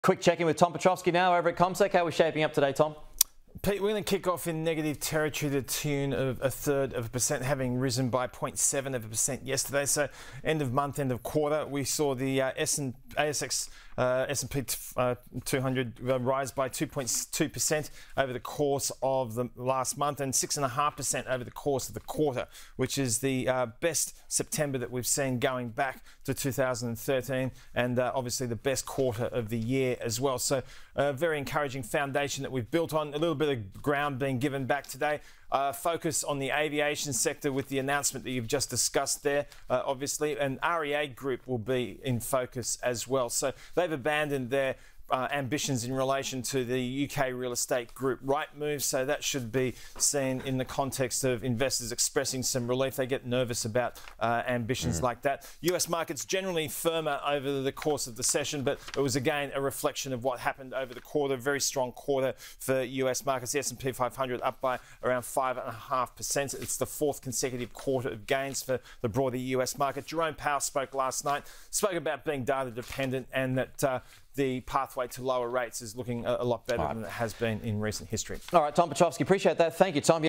ASX 200 expected to kick off in ‘negative territory’ when the markets open
CommSec’s Tom Piotrowski says the ASX 200 is expected to kick off in negative territory today when the markets open.

“We’re going to kick off in negative territory to the tune of a third of a per cent, having risen by 0.7 per cent yesterday,” he told Sky News Australia.

“So, end of month, end of quarter.”

Presented by CommSec.